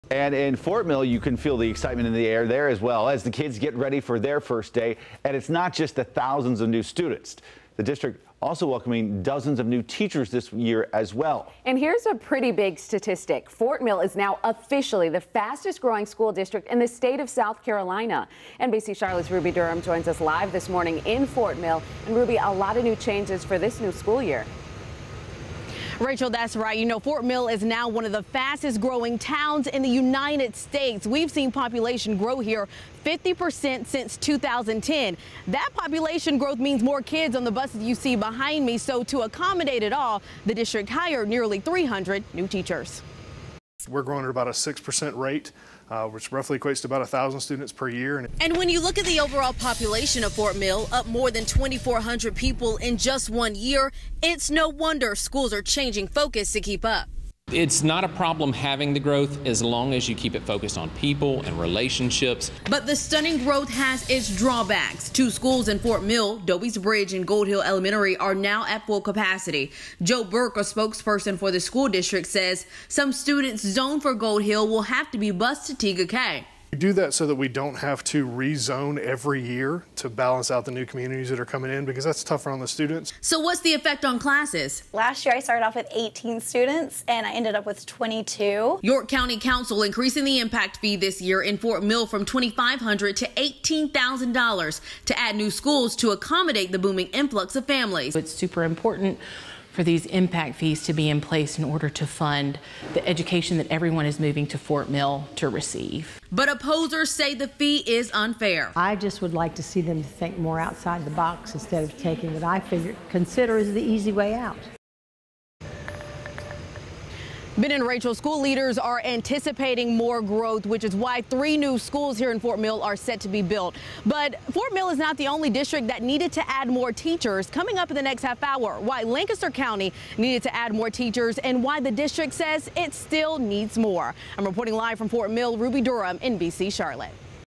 Fort Mill Schools the fastest-growing district in South Carolina
Fort Mill Schools head back to class Monday. And it's not just the thousands of students. Dozens of new teachers are gearing up for their first day of school.